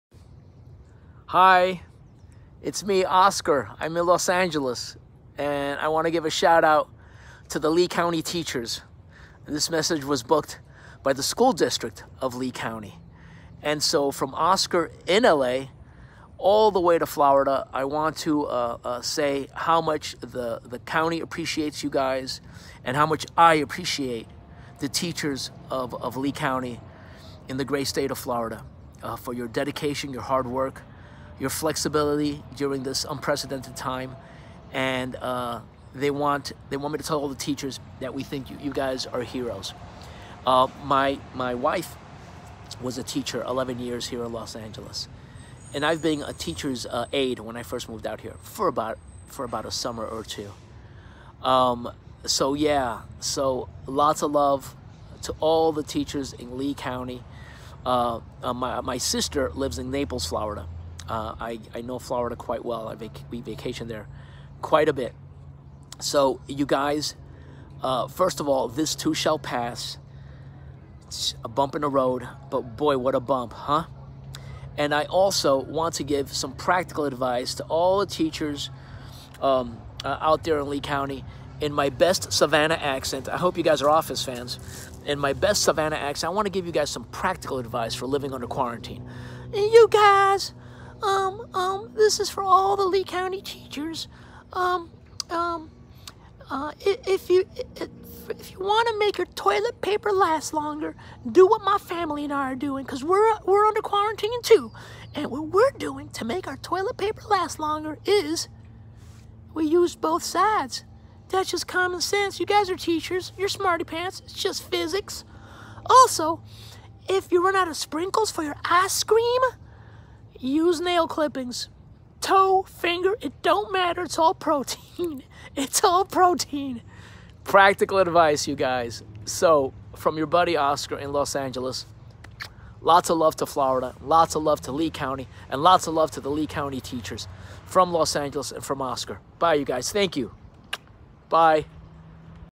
Oscar Nunez has a special message for Lee County teachers!
It’s National Teacher Appreciation Week and Oscar Nunez, star of "The Office" has a special message for our amazing Lee County Teachers!